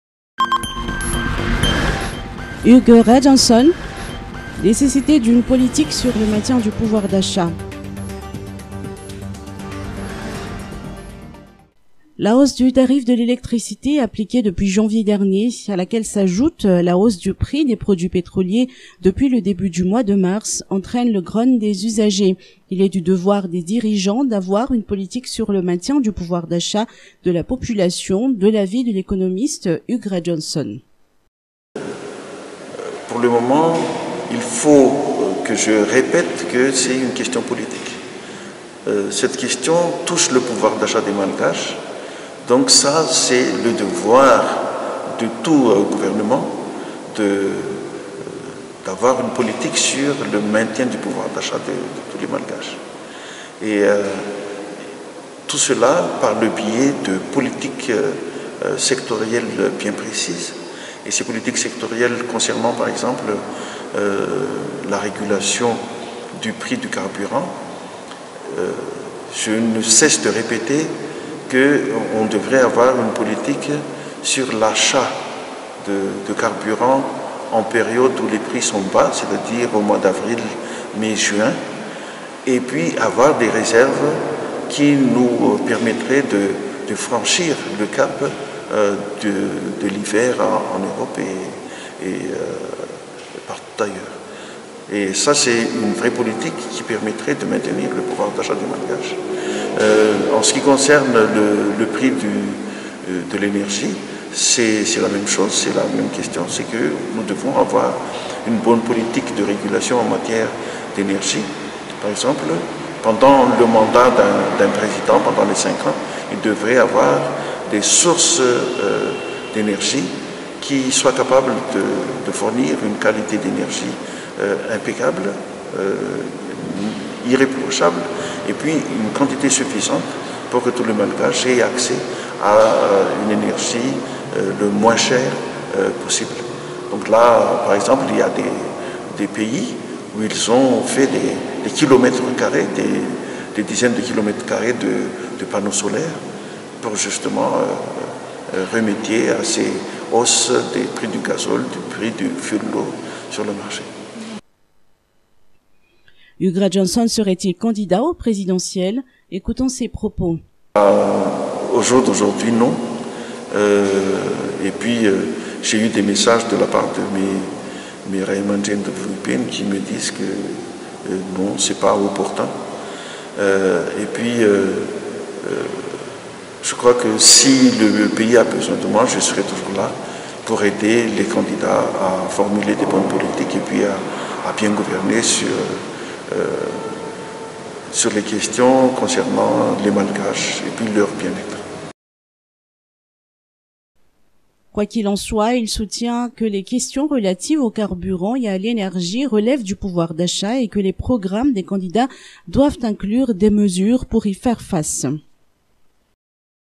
huges
Il est du devoir des  dirigeants d'avoir une politique sur le maintien du pouvoir d'achat de la population de l'avis de l'économiste Hugues Rajaonson.